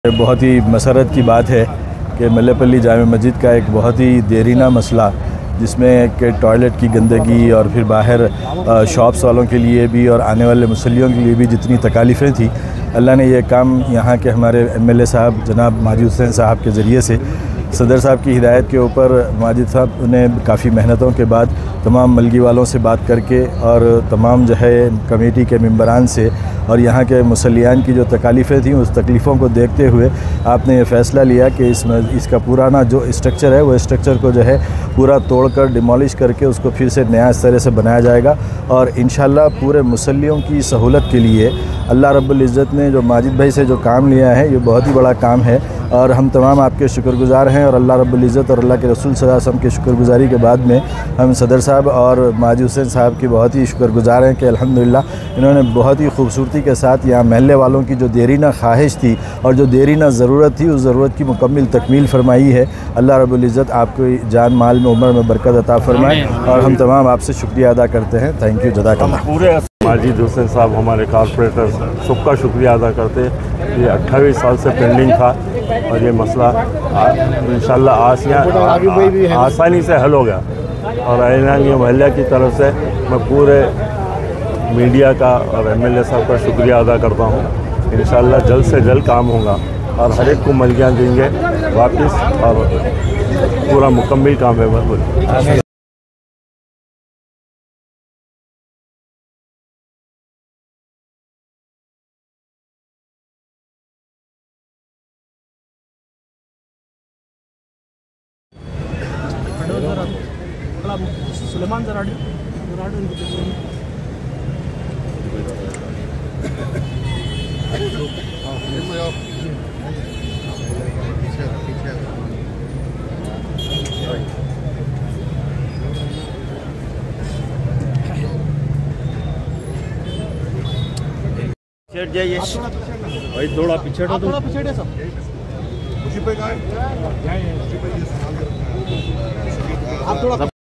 Mufti Jamal-ur-Rahman Ne Rakha Mallepally Badi Masjid Ke Kaam Ka Sang-e-Buniyaad, MLA Majid Hussain
Mallepally Badi Masjid Ke Dukanaat Aur Wazu Khana Renovation Ke Liye Aaj Mufti Jamal-ur-Rahman Ne Sang-e-Buniyaad Rakha Is Mouqe Par Nampally MLA Majid Hussain Bhi Moujood The...